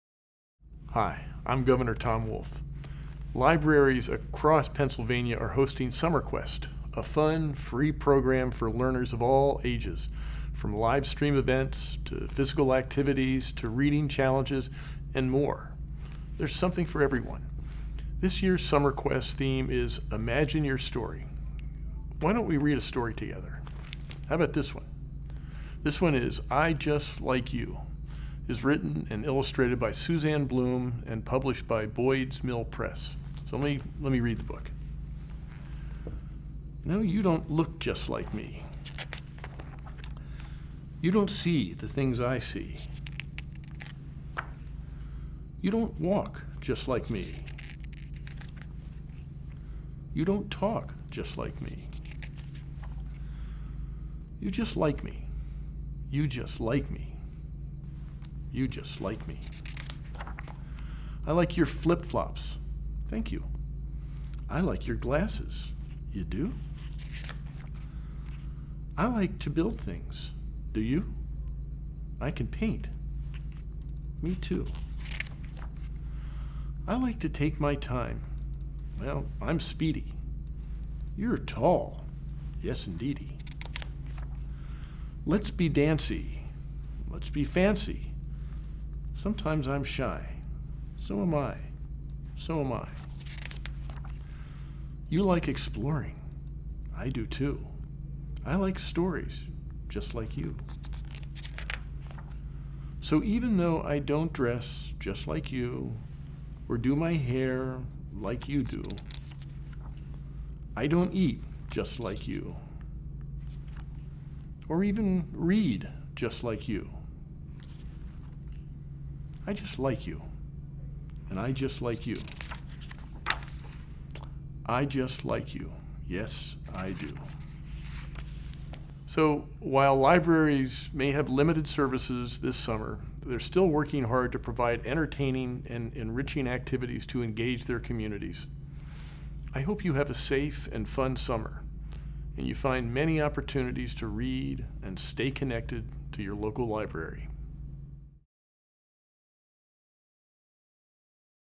Story Time with Governor Wolf: "Imagine Your Story" with Pennsylvania Libraries.
Story Time with Governor Wolf! Governor Wolf reads the picture book "I Just Like You" by Suzanne Bloom, and discusses summer reading programs at Pennsylvania Public Libraries.  It's an invitation to "Imagine Your Story" with programs like our library's "Summer Quest" virtual reading challenge.